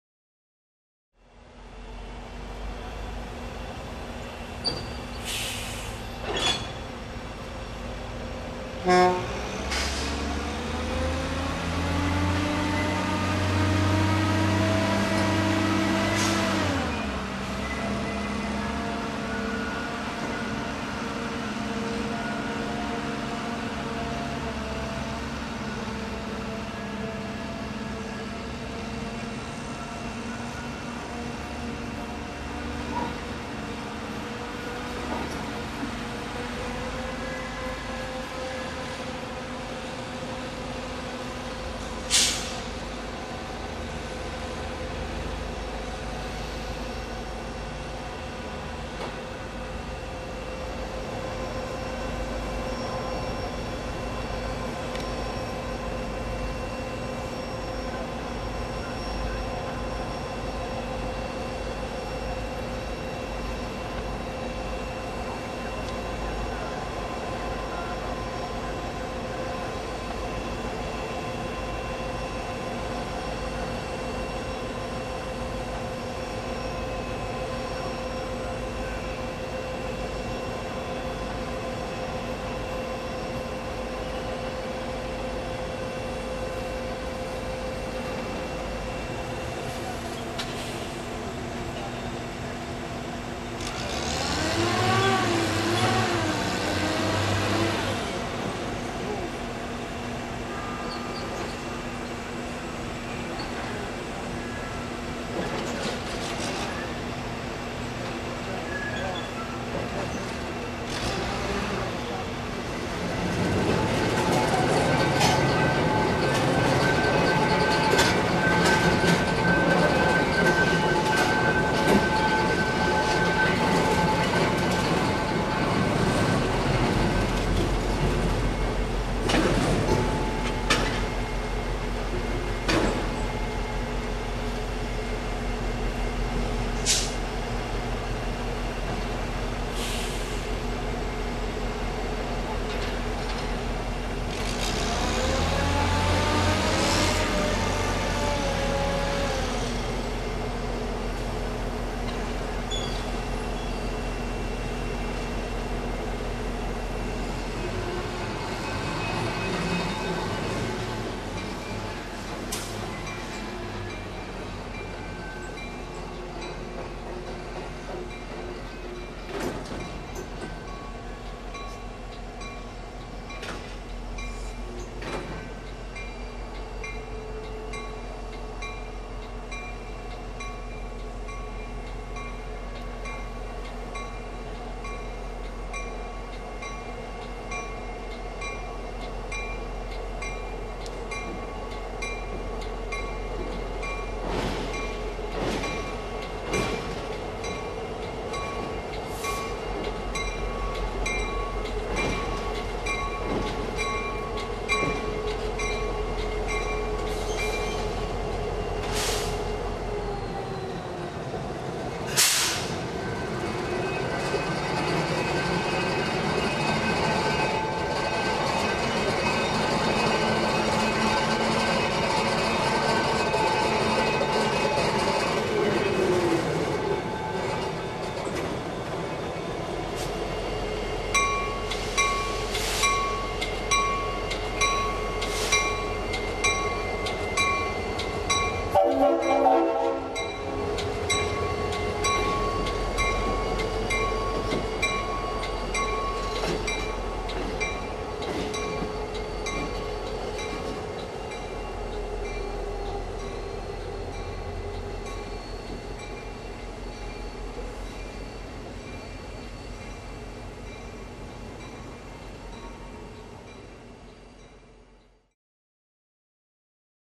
Altoona PA 08.17.17: Last-Minute Shopping
The light was fading fast, but we weren't quite done yet.  On our way back from Tyrone, we stopped in Altoona to see what kind of odds and ends were hanging around behind the Juniata Shops.  I was anticipating nothing more than a cursory look around, but the camera remained at the ready just in case.  That readiness came in handy when we arrived just as the shop's railcar mover was spinning on the turntable with SD70ACu 7236.  With memories of the time I missed a blue SD50 taking a spin lingering in the back of my mind, I had the tripod set and the camera rolling just in time for the mover to tuck the 7236 away on one of the shop tracks.  Resting next to the 7236 was SW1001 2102, a 44-year veteran of Reading and Conrail working out its last days as a shop switcher.  After the railcar mover took a solo ride on the turntable and retreated back to the shop, SD40E 6322 backed in for a ride of its own before shuffling off towards the yard.  Our visit totaled maybe 10 minutes, but in retrospect it couldn't have been timed much better.